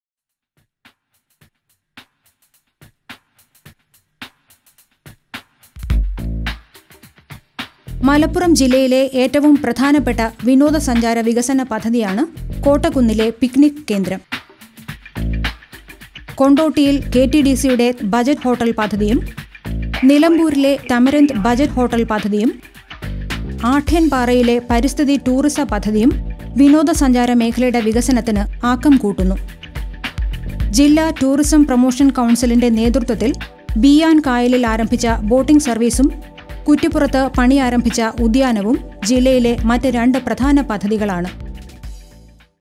Malappuram district, Tourism Projects
Video of Tourism projects at Malappuram district, Kerala. Kottakkunnu picnic spot, Kondotti KTDC budget hotel and Eco tourism project at Adayanpara are the major projects.